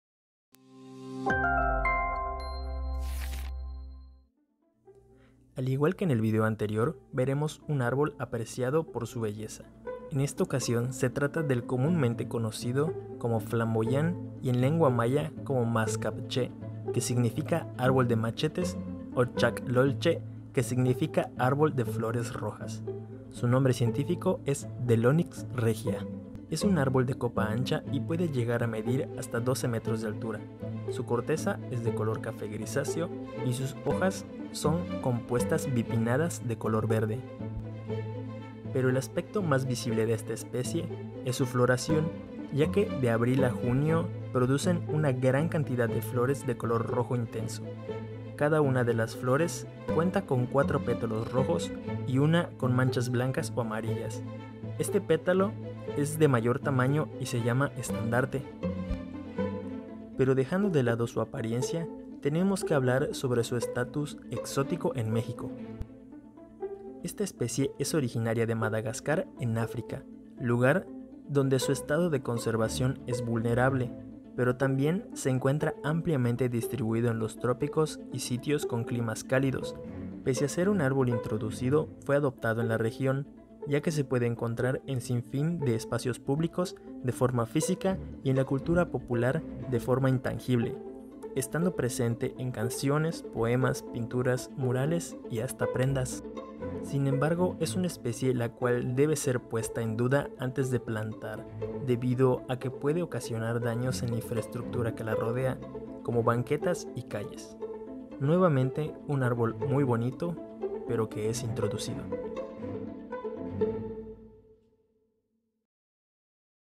Flamboyán - árbol en la península de Yucatán
La especie  Delonix  regia es originaria del continente  africano  sin embargo  tras ser introducida  en  territorio  mexicano y en la península  de Yucatán, con el pasar de los años pasó a formar  parte de la cultura, adaptándolo como propio  de la región